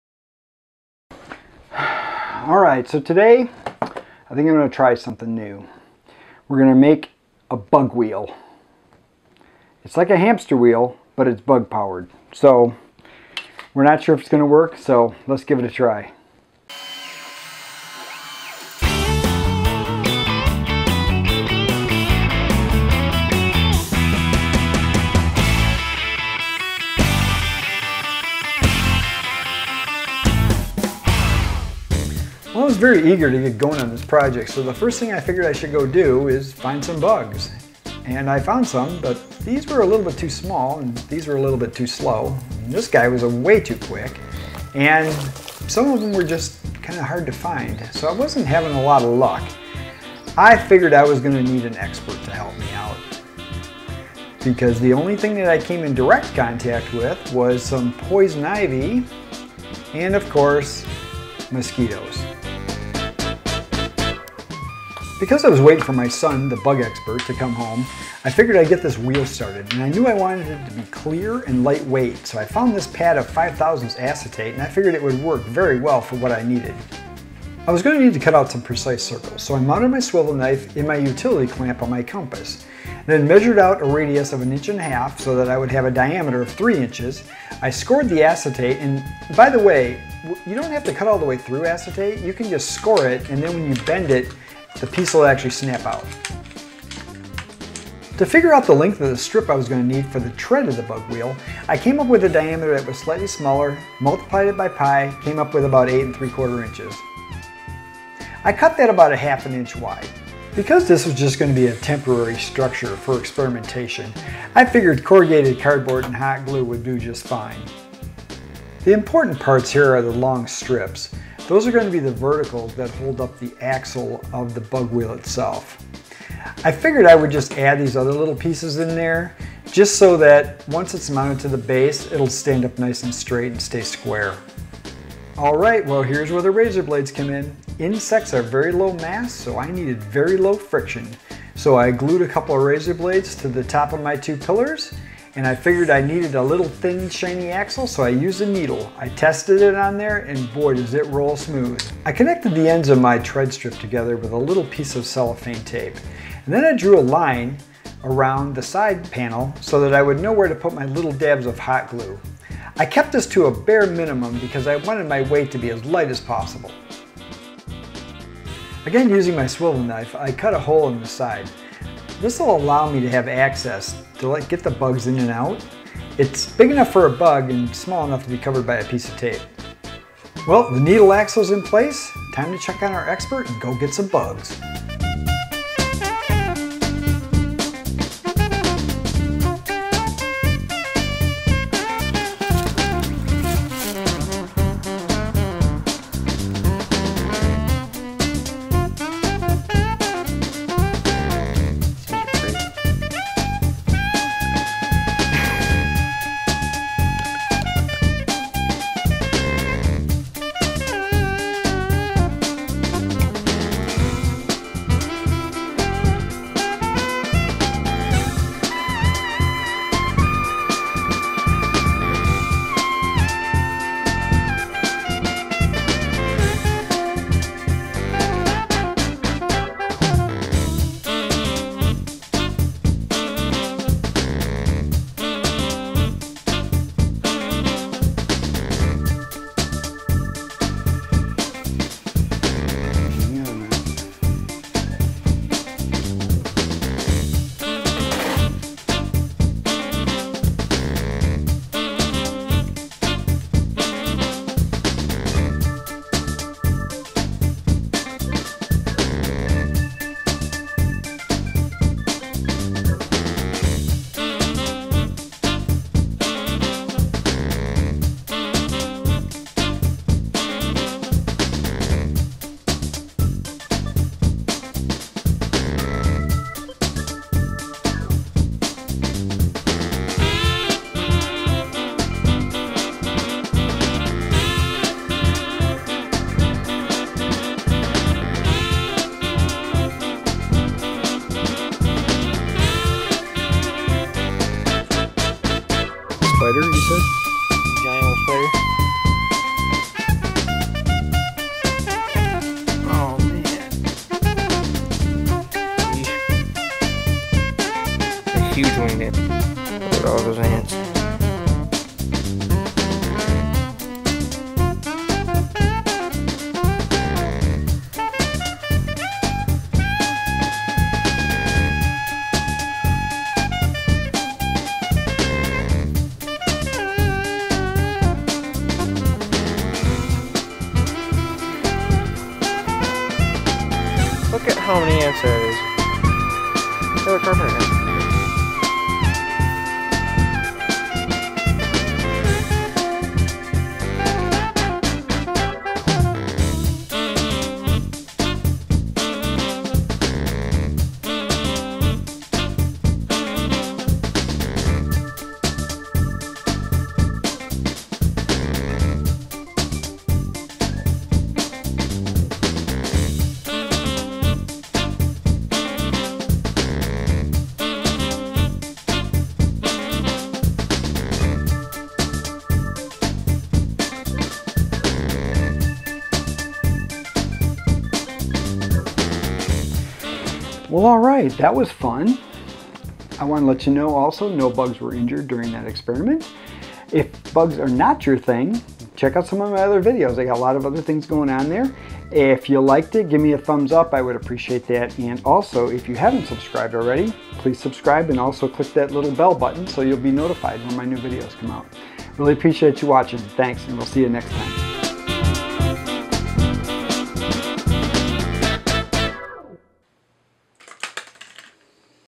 How to Make Bug Experiments - They will Keep Running | Experiments with insects | Physics and Math.
In this bug experiments project, I build a Bug Wheel. This is very much like a Hamster Spinning Wheel, funny to see this common amusement for pet rodents. They will Keep Running! I actually had no idea if it would work when I started to build it. Insects have very small mass, so I knew I needed to keep the structure as light as possible. Their little bodies needed to overcome the mass of the cylinder.

For the first time on my channel, I do a voice over track that includes a few "How To" details. The razor blades are used to provide a minimal friction surface for the needle working as the axle.

This is not intended to be a step-by-step project by any means. This can be a fun project for kids, but i would suggest using something other than razor blades as bearings. I have not tested it, but I think aluminum foil may work by poking the needle through a few layers.

I also introduce a couple of vintage drafting tools that I dug out from storage. These are from back in the day when I used to do airbrush illustration work. The sheets of acetate are more than 20 years old as well. The swivel knife is no longer available. If I can find links to these tools at a later date, I will post them below.

Tools:

• Utility Knife
• X-acto Knife
• Razor Blades
• Hot Glue Gun

Art Supplies:

• Acetate Shetes
• Black Markers

Camera and Video Gear:

• Main Camera
• Main Lens
• Camera Frame
• Secondary Camera
• Lighting Setup
• Magic Arm A
• Magic Arm B
• Video Monitor